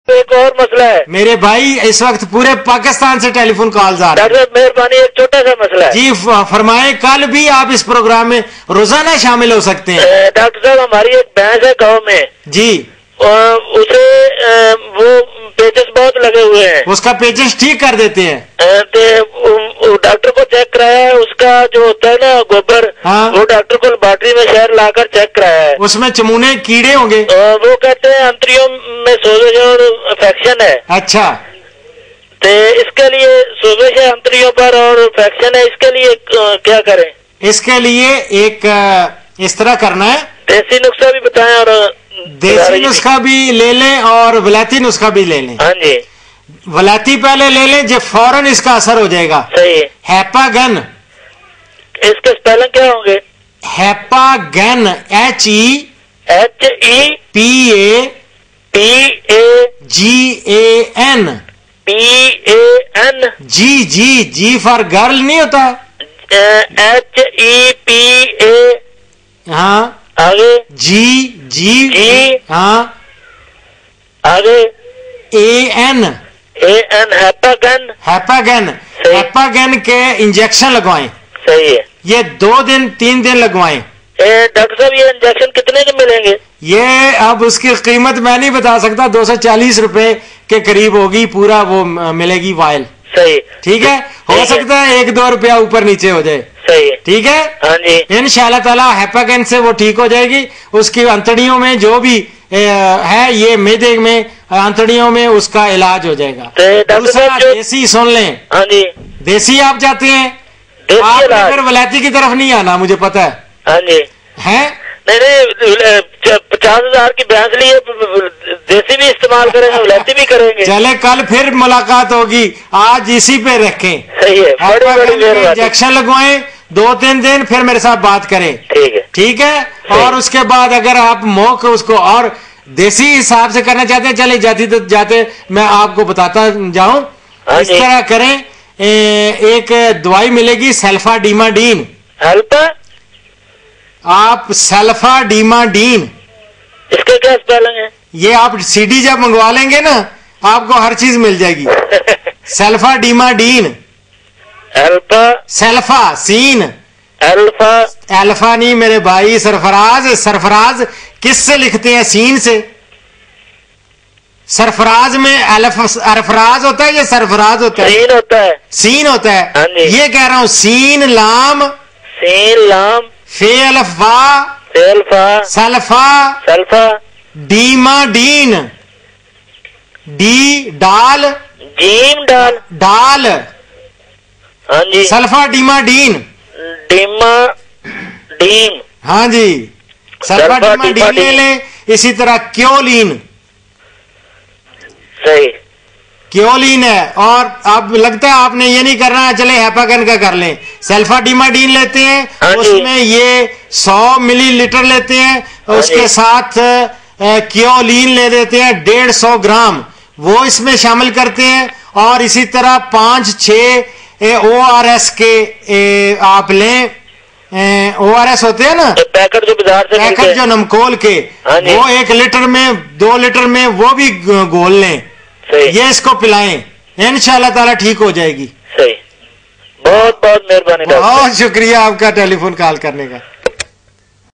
Intestinal infection to buffalo in Sahiwal Pakistan Dr. Ashraf Sahibzada
Dr. Ashraf Sahibzada Chairman Noble Deeds foundation Islamabad Pakistan, renowned scientist replies quarries of farmers on help line. Currently Helpline # is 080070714 and Cell # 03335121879